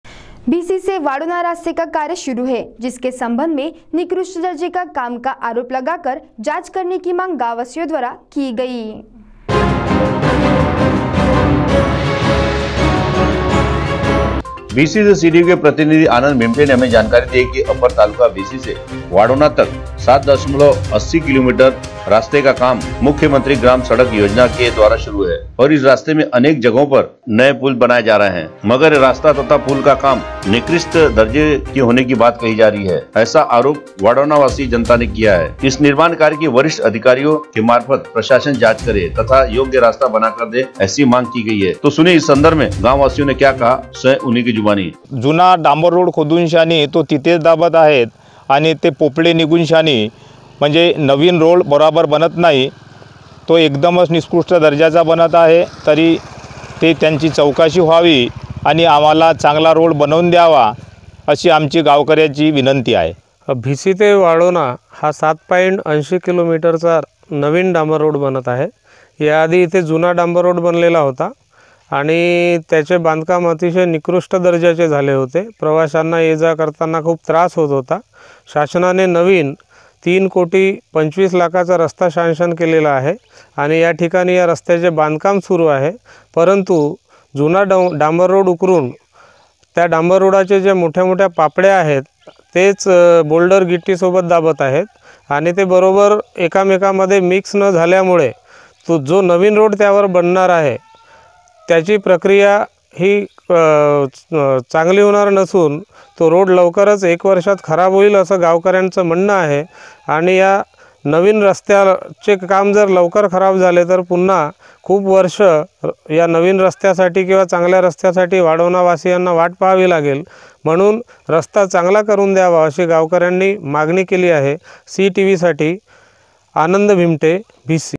भिसी से वाढोणा रास्ते का काम निकृष्ठ दर्जे का होने की बात नागरिकोने की
CTV News 29 JAN 2020